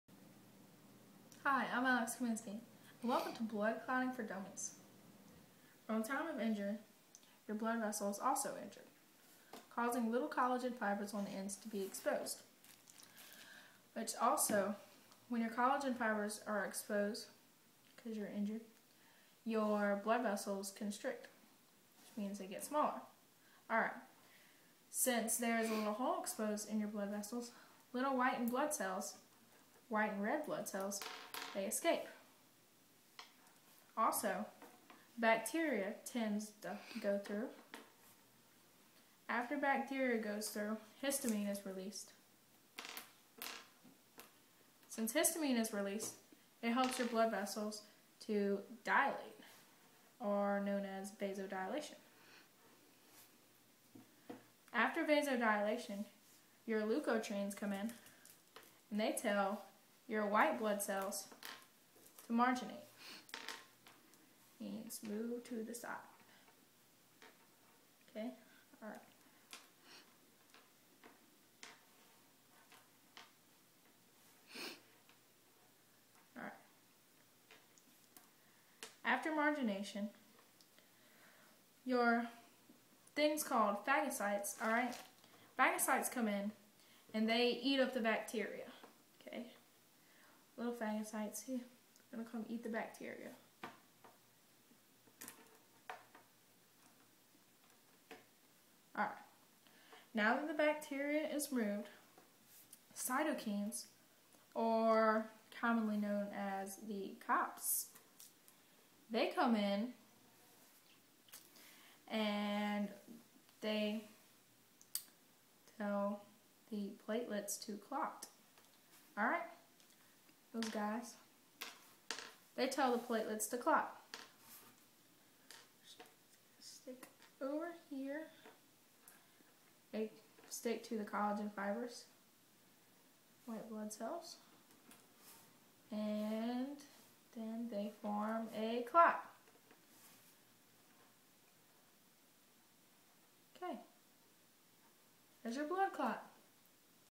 Blood clotting for dummies
This is just the basics of blood clotting it was a project for a 10th grade sports medicine class